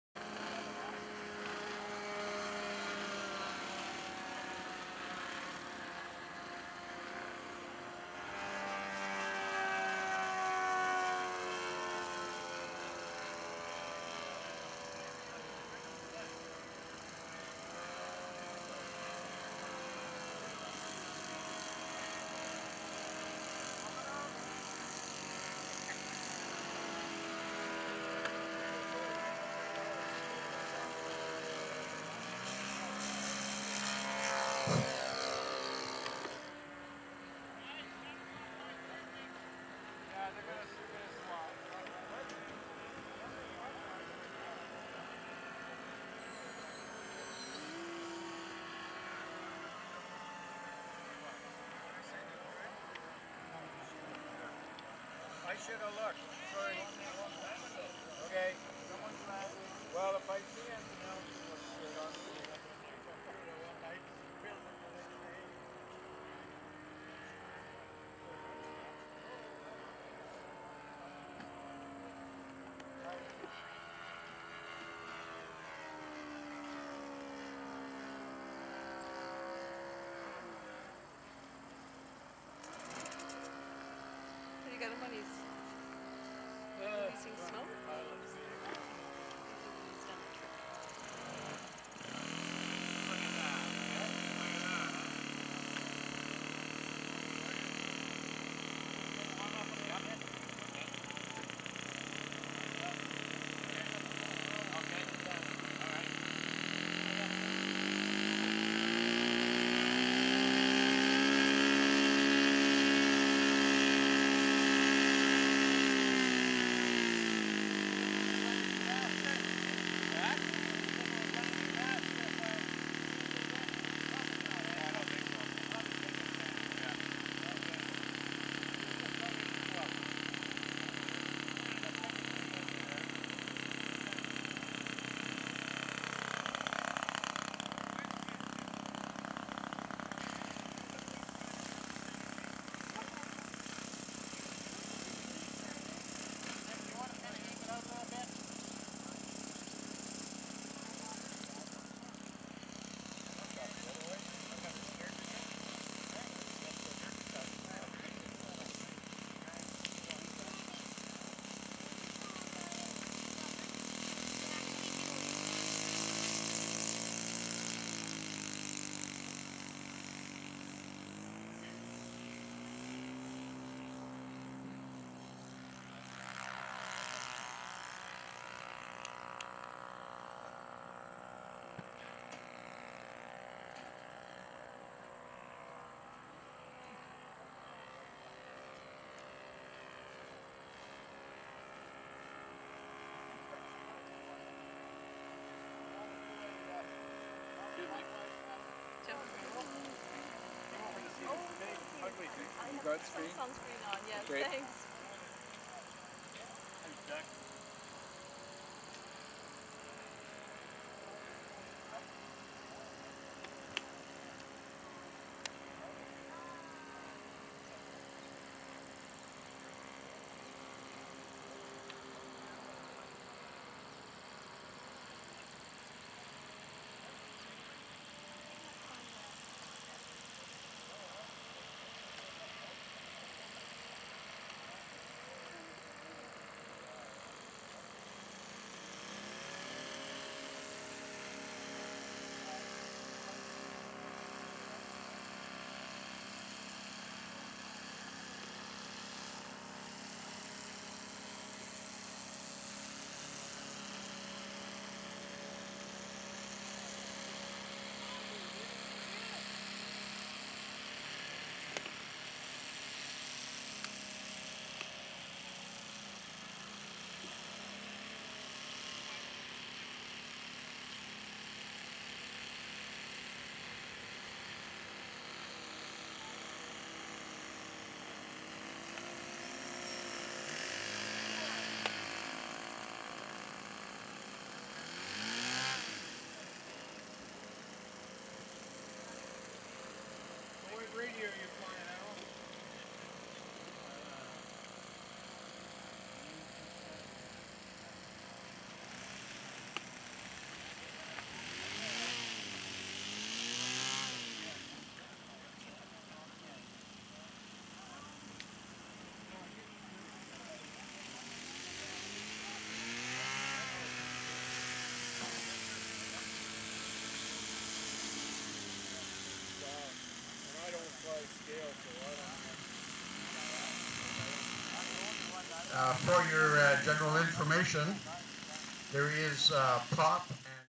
Vernon RC Aeromodellers - Labour Day Fly In 2010
Vernon RC Aeromodellers - Labour Day Fly In 2010. Check out the fantastic assortment of airplanes at the Vernon RC Aeromodellers air field.